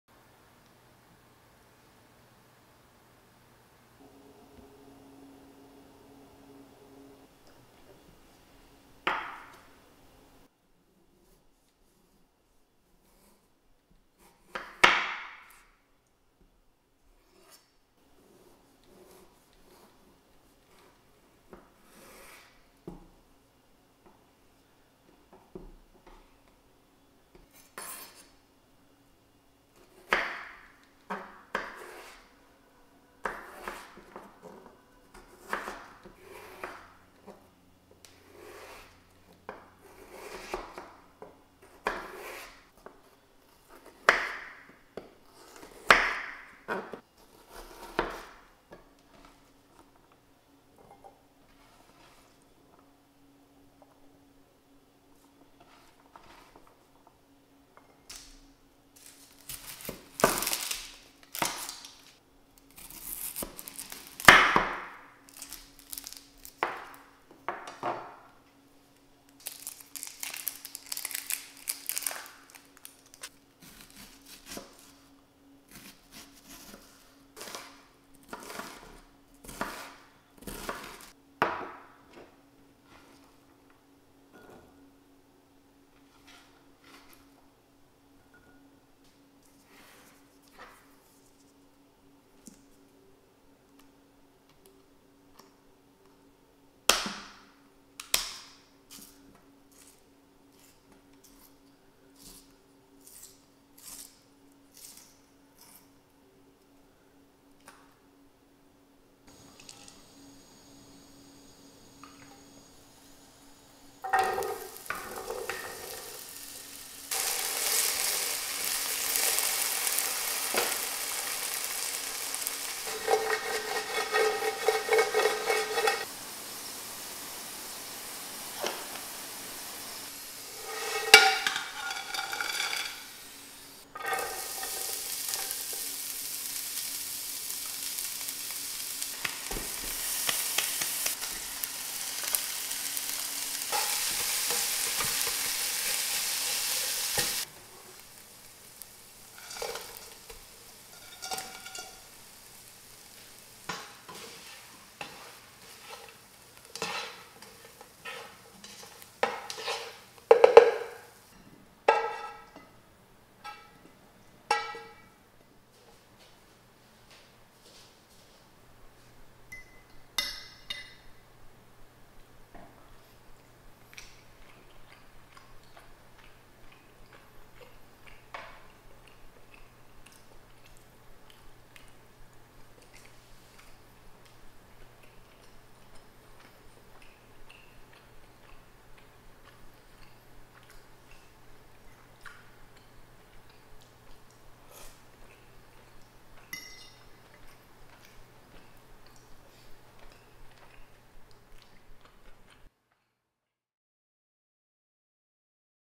COOKING ASMR, Corned Beef Hash, Cooking & Eating Show
Today we are cooking and eating corned beef hash.  This is a really simple and easy recipe

1 Diced Russet Potato
Pinch of Salt
Half of a White Onion
1 Can of Corn Beef